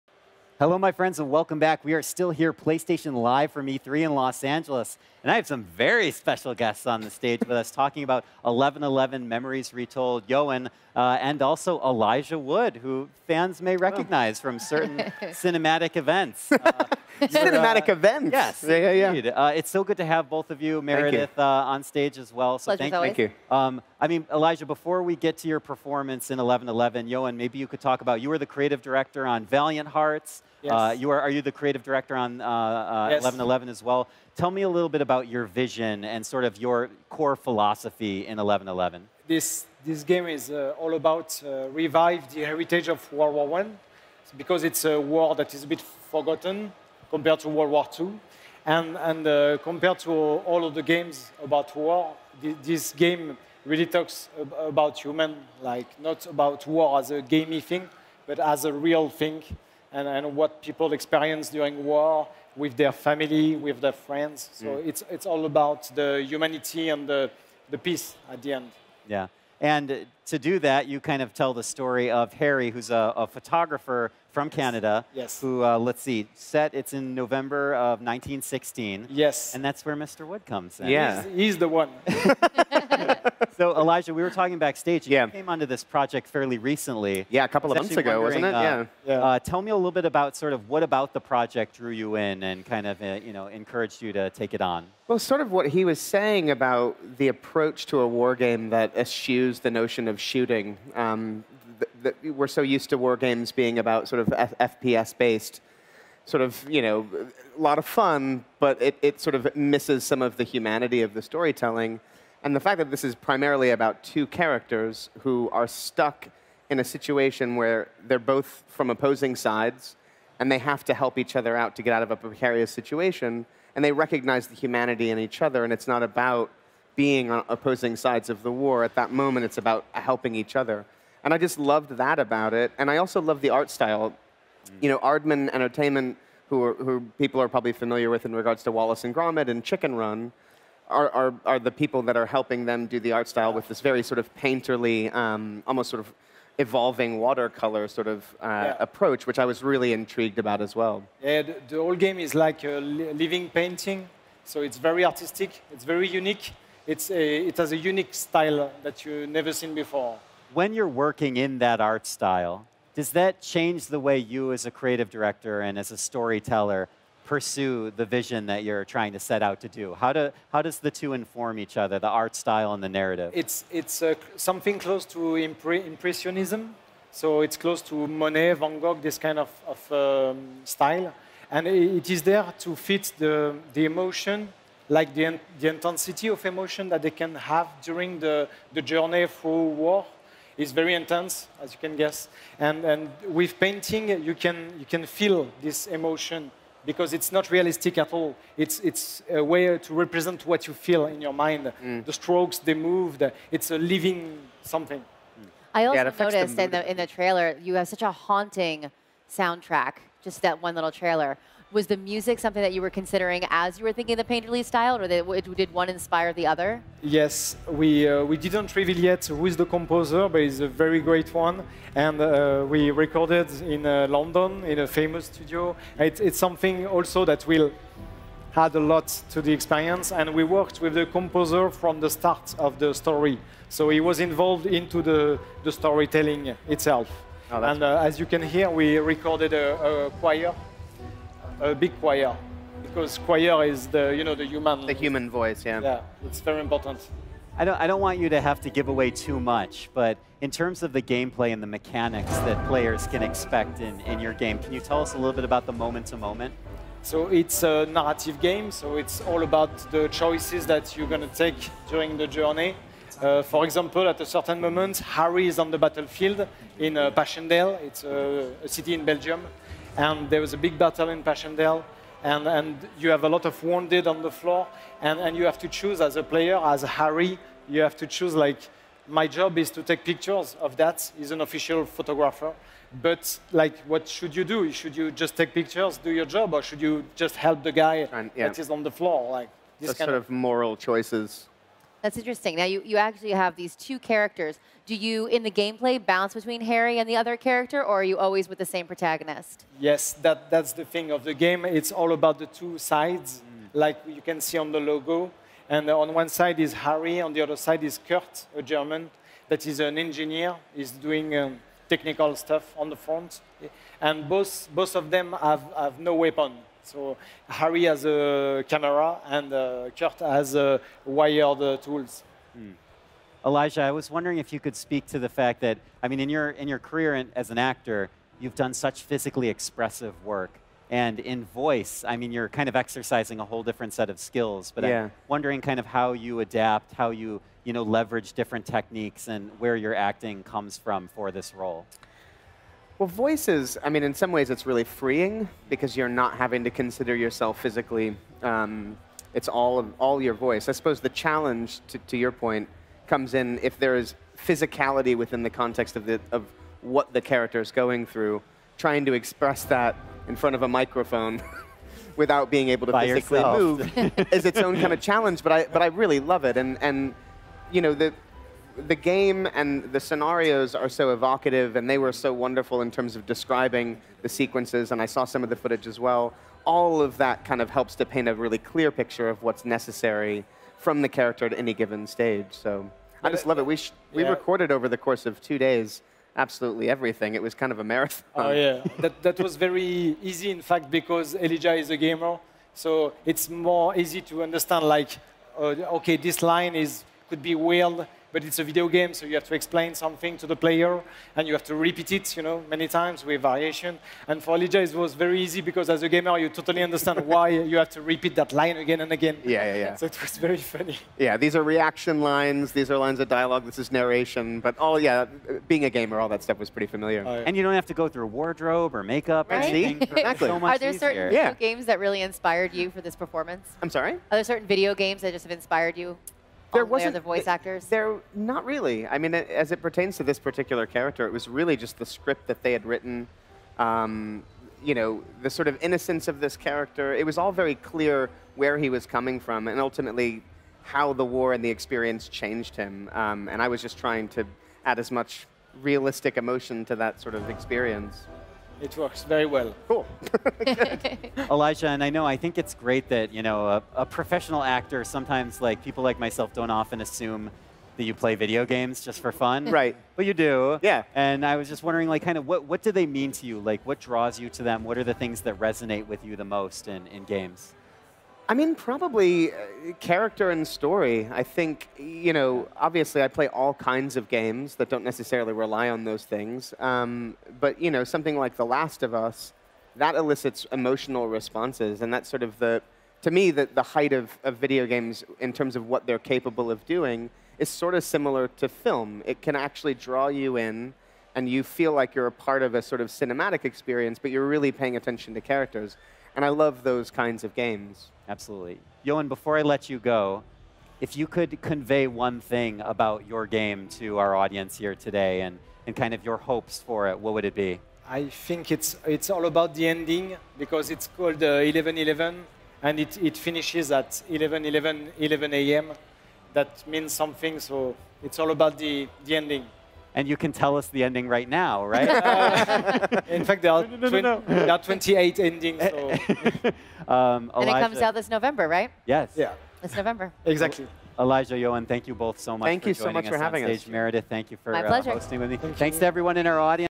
MEMORIES RETOLD Elijah Wood & Developer Interview - Official Sony Live Showcase E3 2018
MEMORIES RETOLD Elijah Wood & Developer Interview - Official Sony Live Showcase E3 2018 This is the interview with Elijah Wood and Developers of Memories Retold shown at Sony's Live Showcase at E3 2018.

We've got walkthroughs from everything from Resident Evil 7 to LoZ Breath of the Wild.